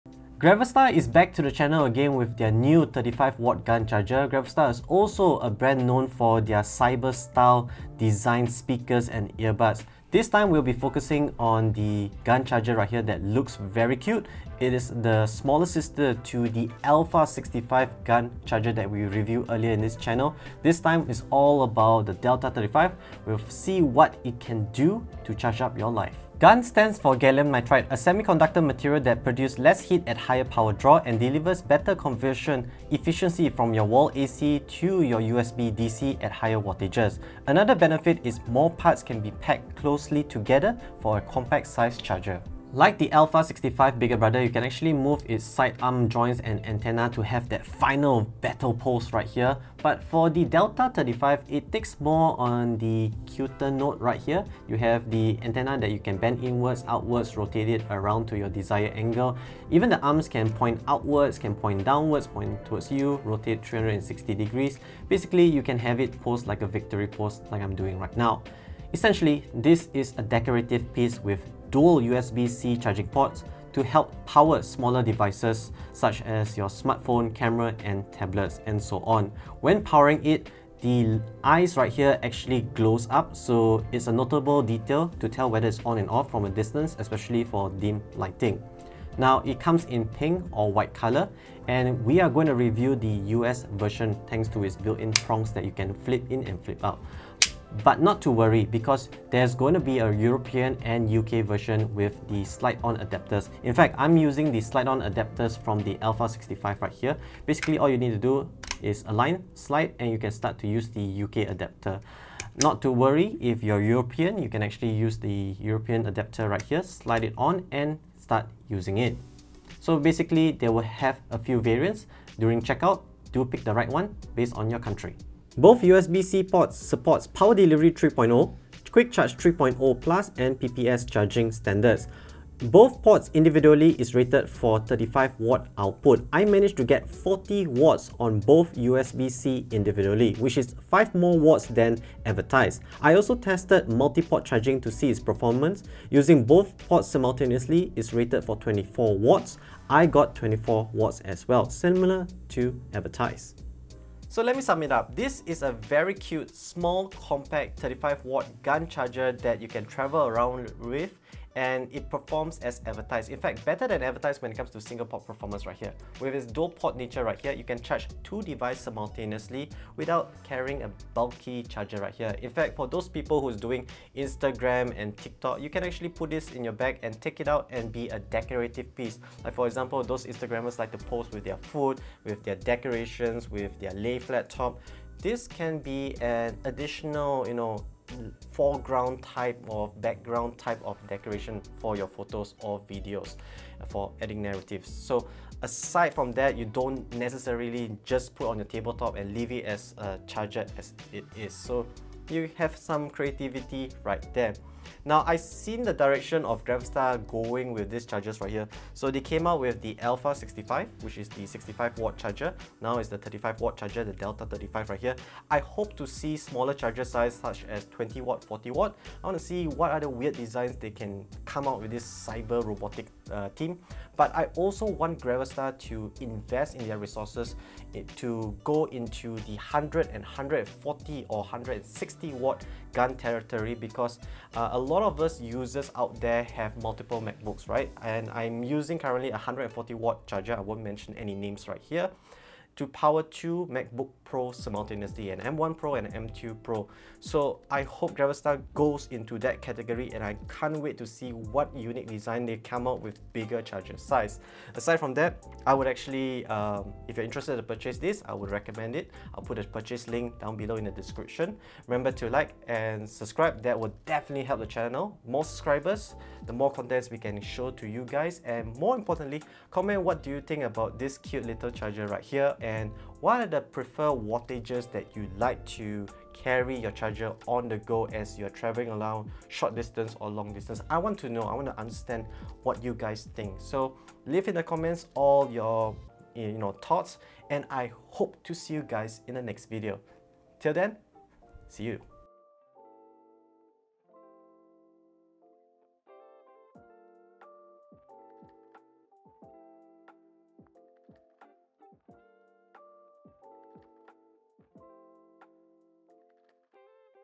Gravastar ALPHA65 - 65W PD GaN Wall Charger
Buy Best Cyberpunk 65W GaN Technology !

Timestamp:
0:00 - Intro
0:36 - Design
3:08 - Charging Performance
3:52 - Multiport Charging
4:44 - Summary

* mecha robot-inspired charger